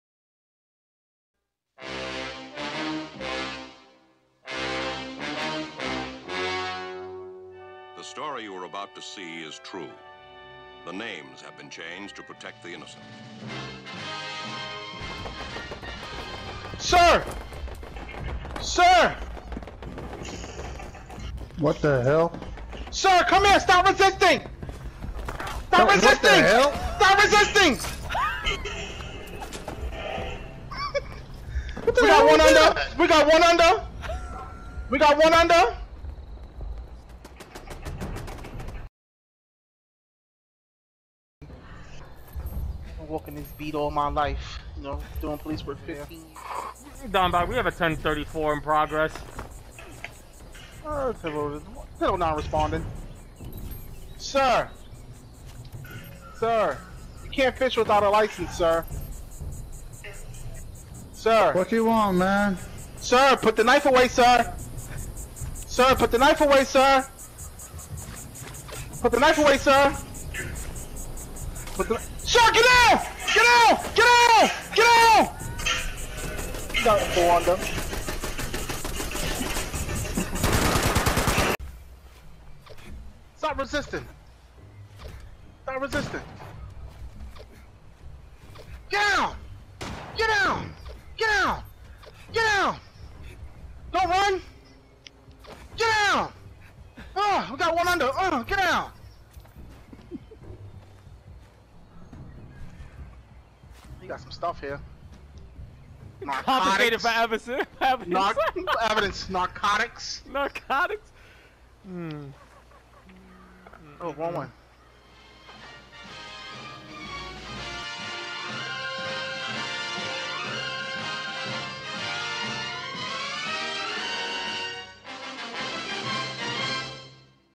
Officer DonBai
Some moments from Rage Chronicles featuring "Officer Don"